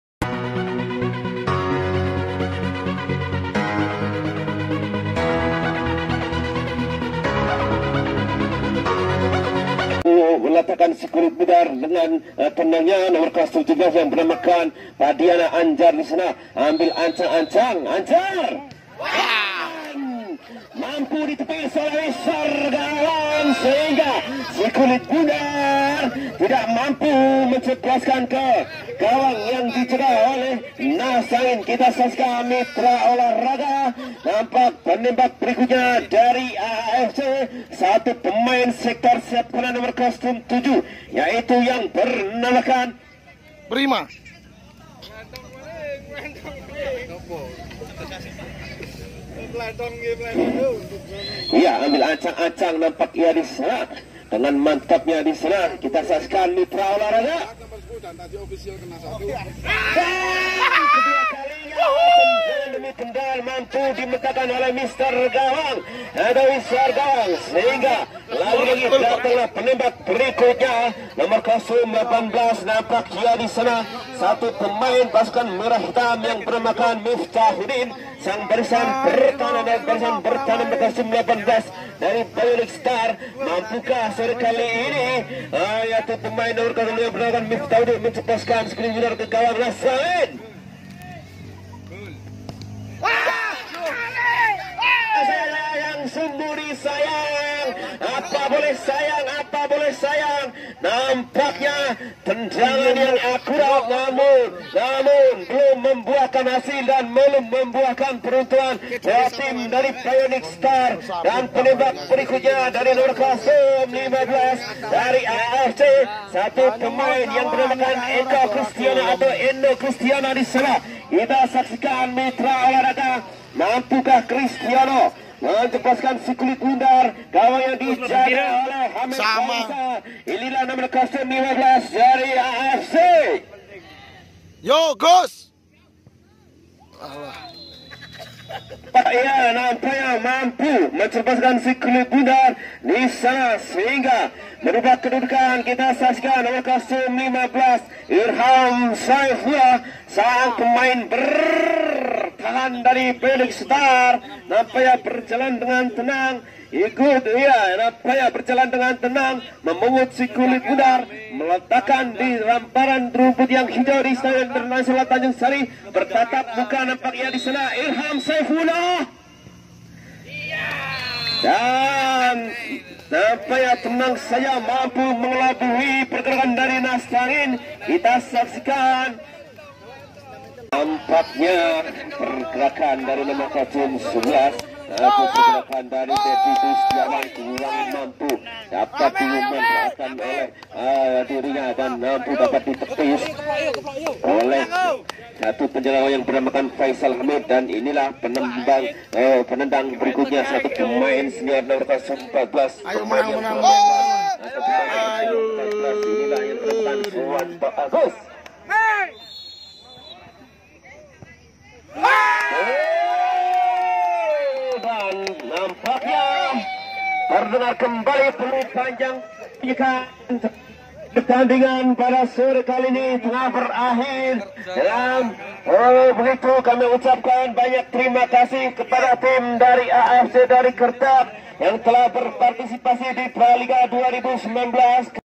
Adu pinalty paling menegangkan pra liga (askab pssi wonosobo zone 4)
Praliga 2019 kembali di gelar oleh askab pssi wonosobo, kali ini salah satu adu pinati di praliga wonosobo 2019 di zona 4 meliputi kecamatan kertek kalikajar sapuran dan kepil, salah satu pertandingan yang harus di selesaikan melalui babak adu pinalty karena skor sama kuat antara bionic star fc glagah vs Anggurgondok fc kertek berakhir dengan kemenangan bionic star fc glagah dengan skor 4-3 melalui adu pinalty